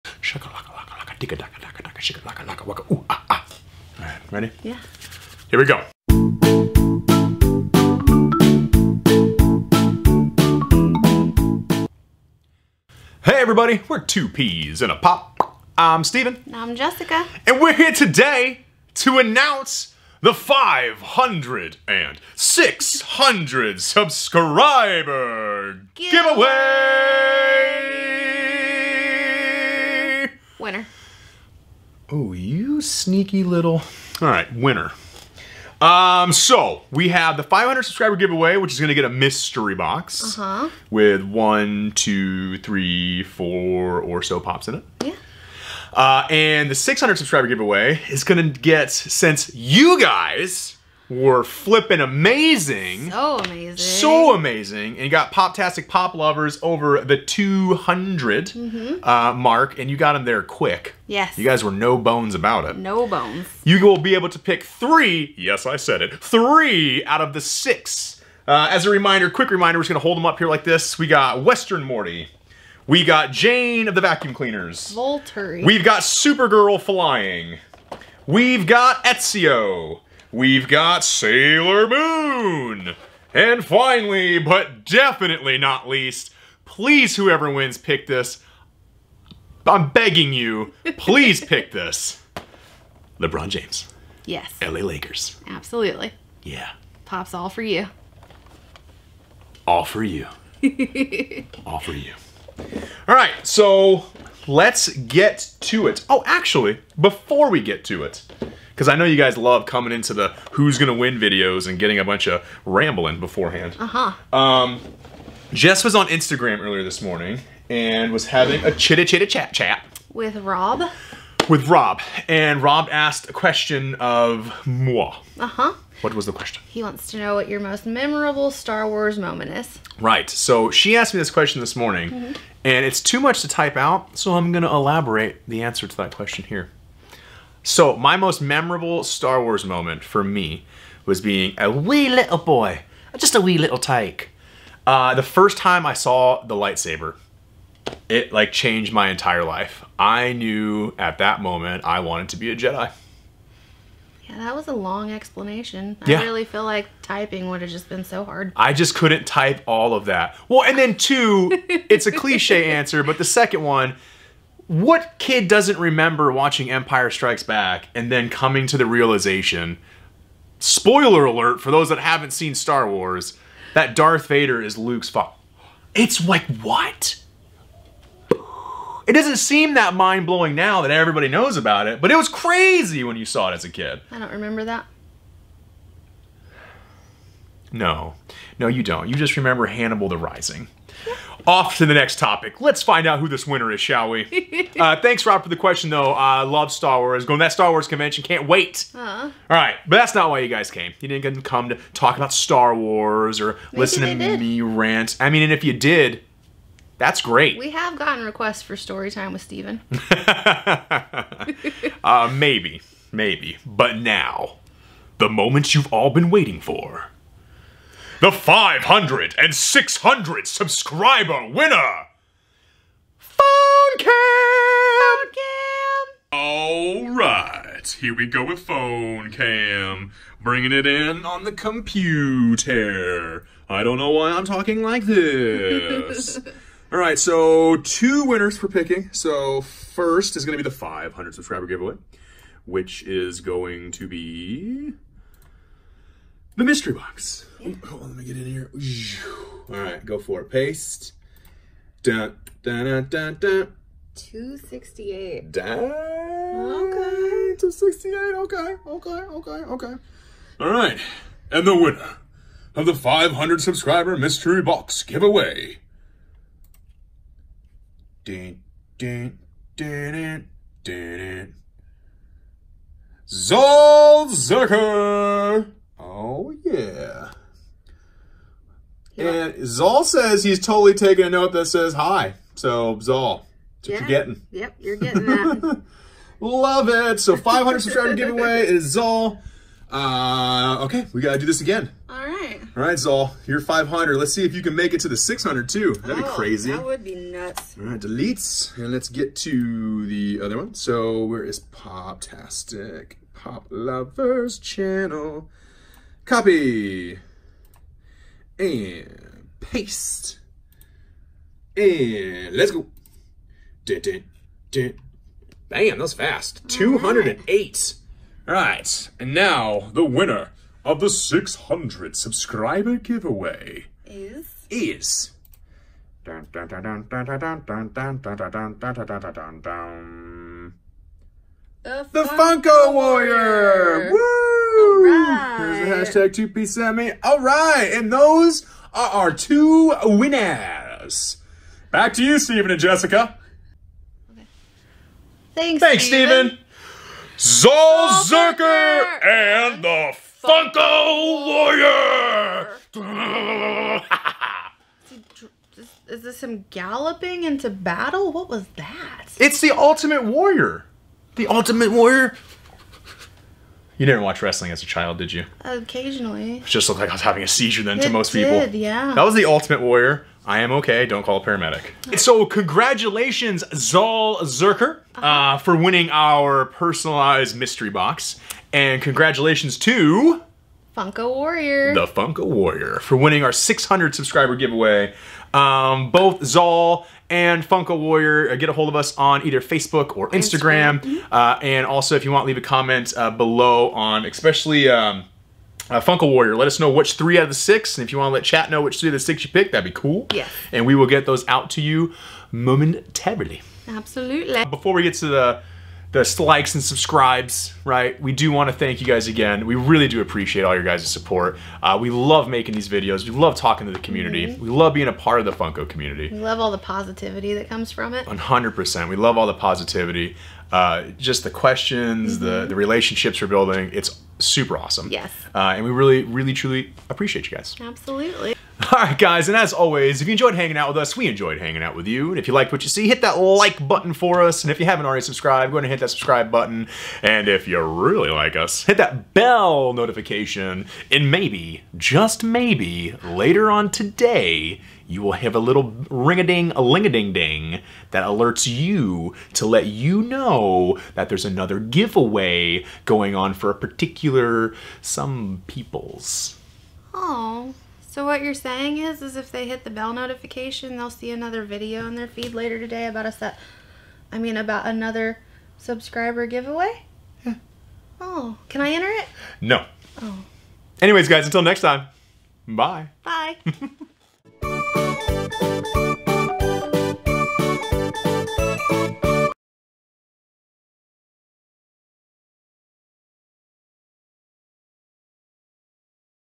500/600 Subscriber Giveaway Winners!!!
It is time to find out who the winners are.

HUGE! We love all of you and really appreciate all of you!

If you want to send us letters, gifts or goodies, please send to:
2 Peas in a Pop!

Logo created and designed by Cory (CompletelyCORY)